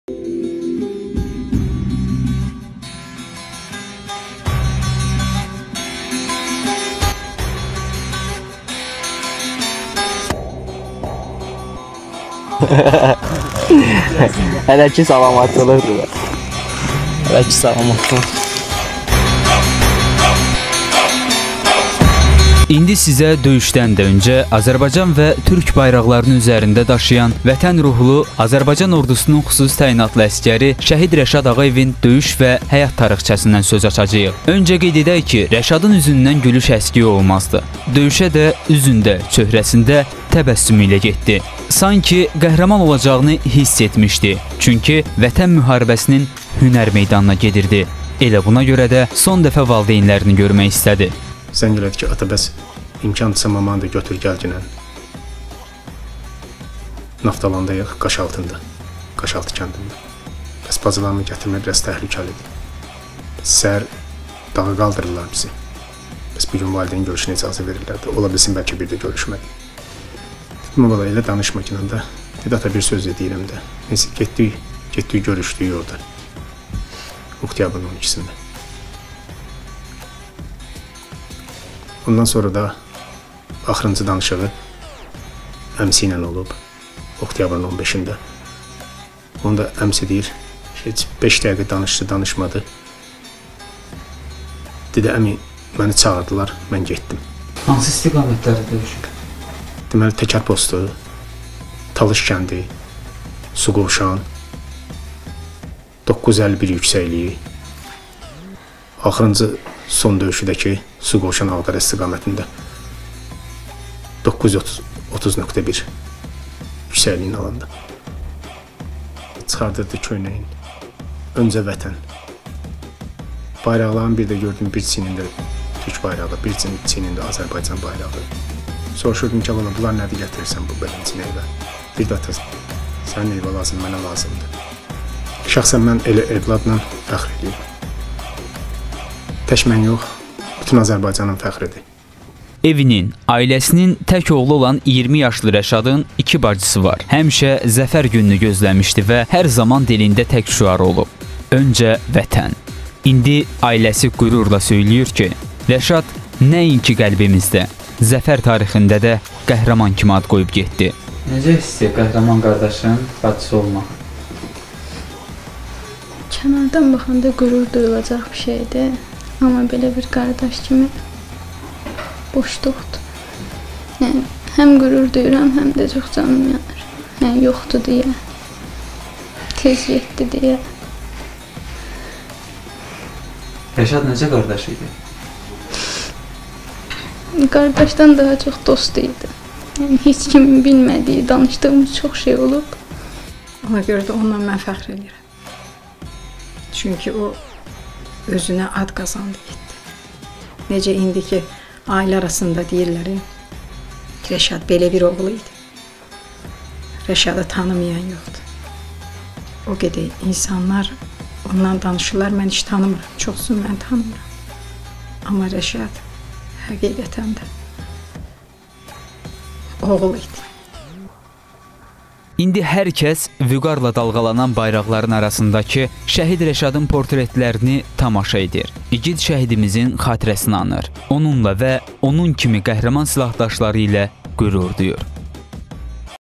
20 yaşlı xüsusi təyinatlı şəhid əsgər
Şəhid Rəşad Ağayevin döyüş tarixçəsi